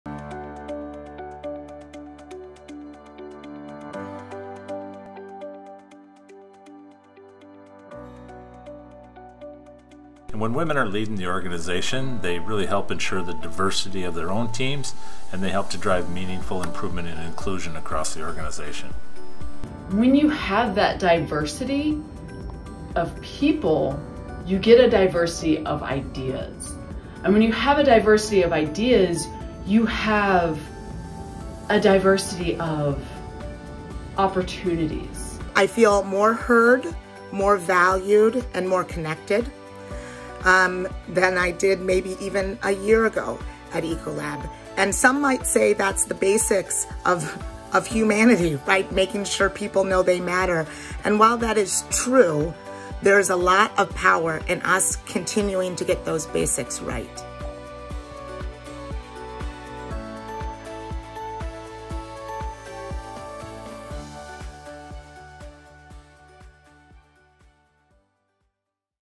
International Women's Day 2022: Supporting and Empowering Women
Ecolab connected with member of E3, our employee resource group focused on empowering and accelerating the advancement of women in the workplace, to learn what they're doing to break the bias and challenge the way we view women in the workplace, at home and in our communities.